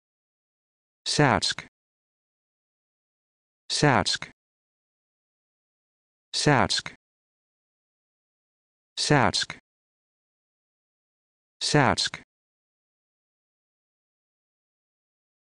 How to Pronounce Tsatske
This video shows you how to pronounce Tsatske